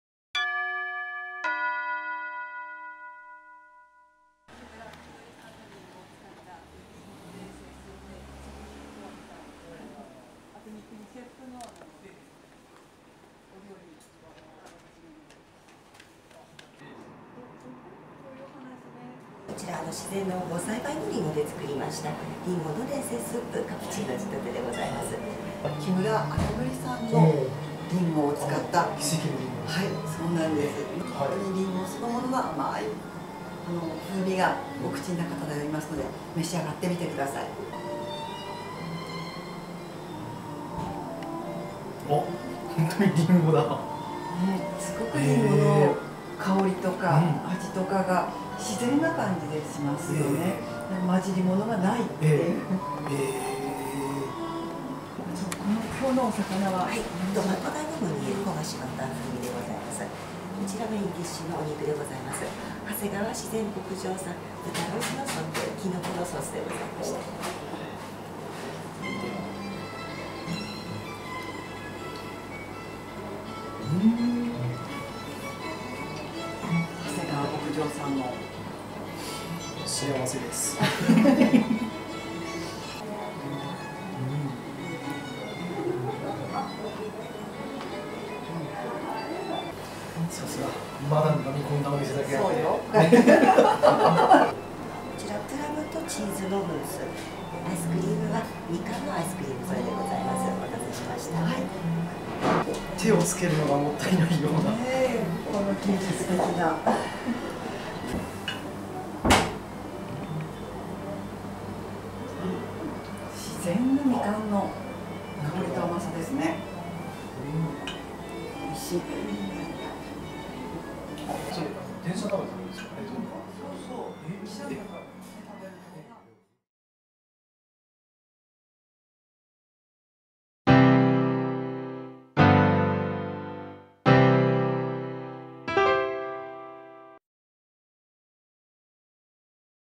弘前街歩き（マダムとデートその４ レストラン山崎編）
弘前のガイド団体「ふれ～ふれ～ファミリー」の一條敦子さんと弘前市内を案内します。ランチをいただいた「レストラン山崎」さんは、地元の厳選された無農薬の食材を使ったフレンチのお店。奇蹟のりんごで有名な木村秋則さんのりんごや長谷川自然牧場産の豚肉を使用した料理は絶品です。